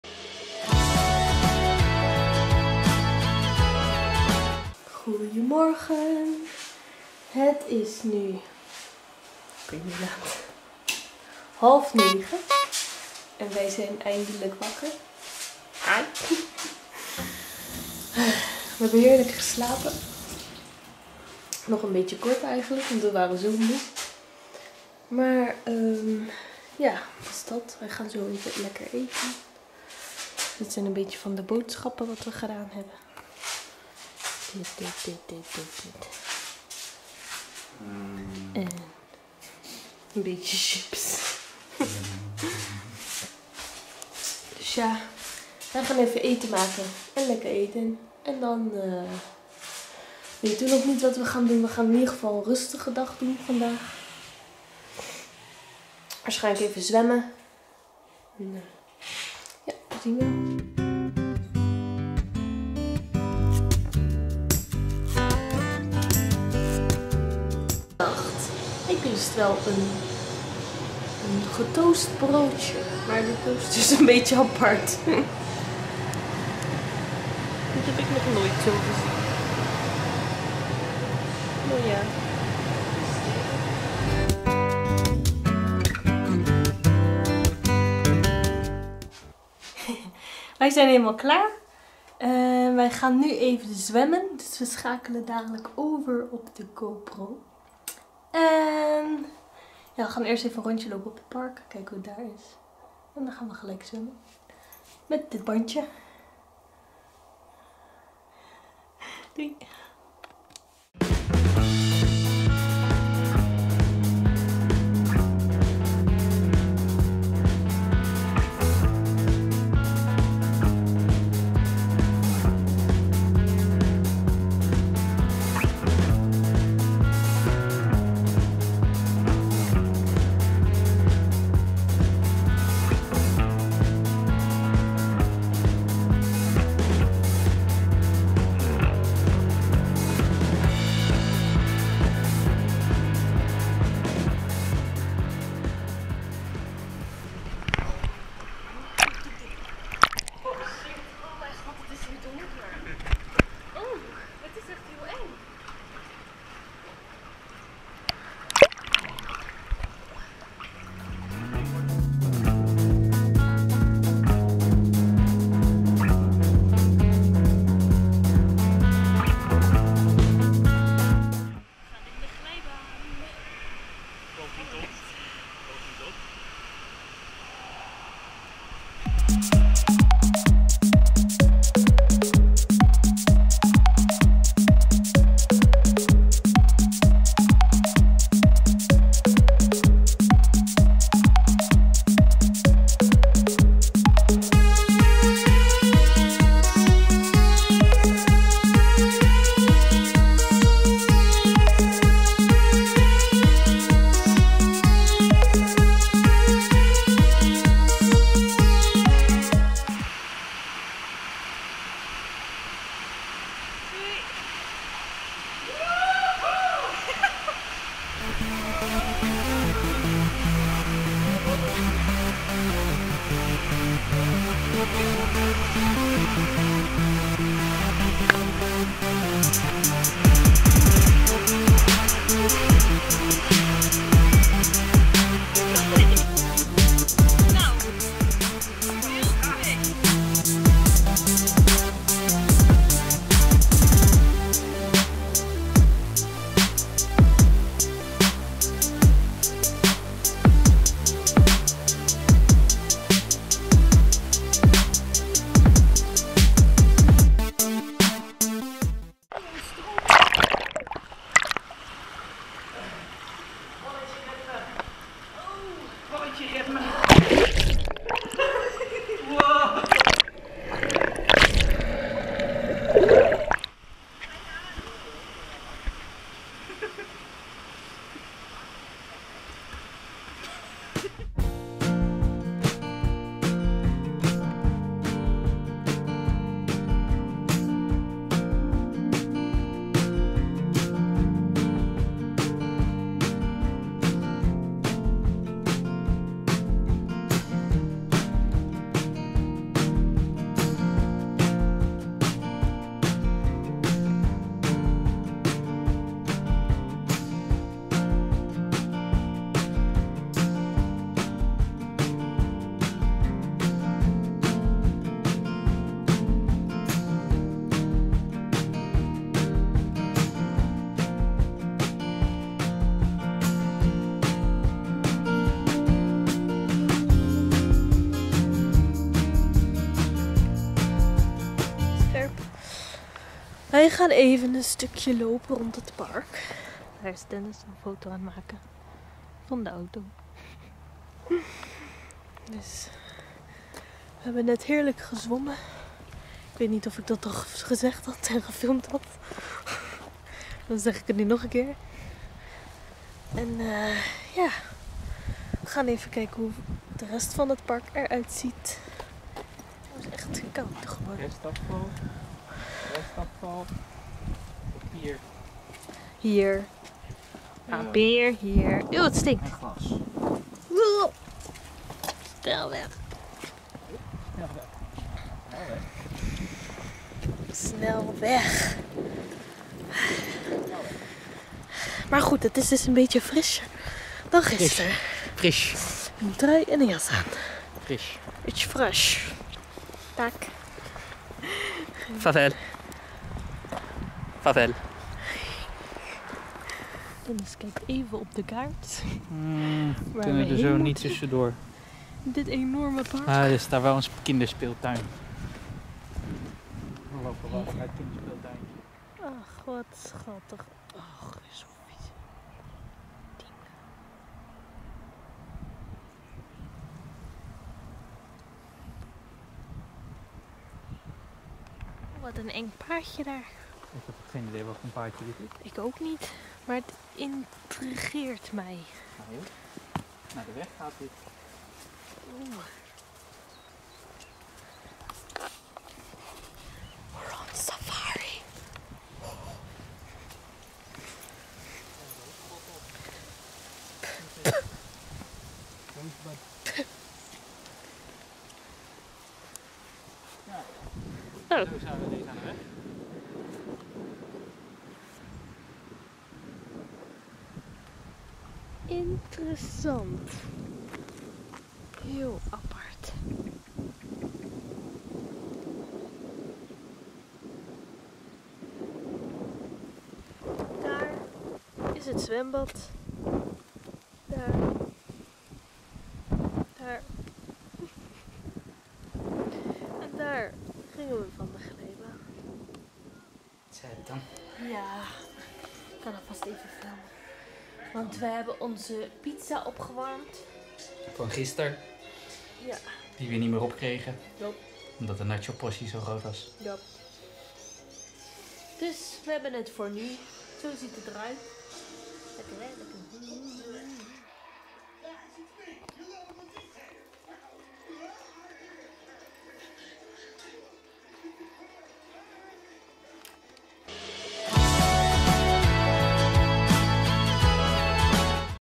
VLOG #10 | Denmark DAY 2 | Zwemmen met de GoPro! | Swimming with the GoPro!
*With English Subtitles | Select the subtitles*

Op dag 2 gingen wij zwemmen en nam ik de GoPro mee!
Op 18 September 2017 begonnen wij onze reis van 8 uur naar Denemarken. Ik heb onze hele vakantie gefilmd en deze komen nu per dag/vlog op mijn kanaal!
On day 2 we went swimming and i took the GoPro!
On September 18th we started our trip 8hour trip to Denmark. I filmed our entire holiday and these are coming per day/vlog on my channel.